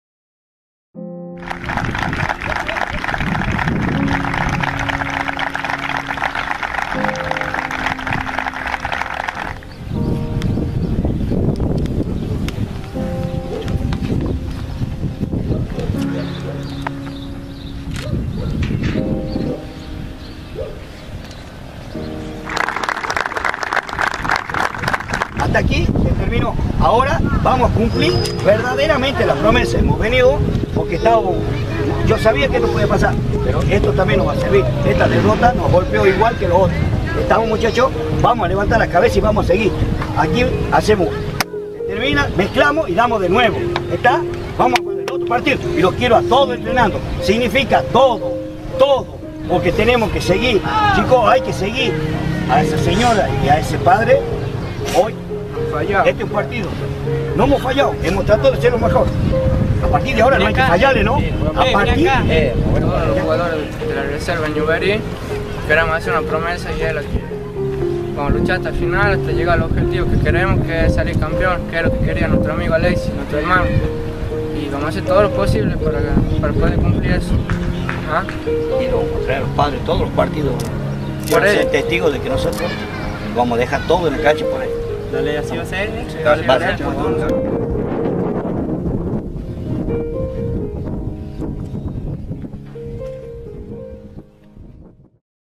La promesa de la Reserva de Jorge Newbery
Los chicos de la Reserva de Jorge Newbery disputaron su primer partido sin Alexis; estuvieron por sus padres a quienes le hicieron una promesa.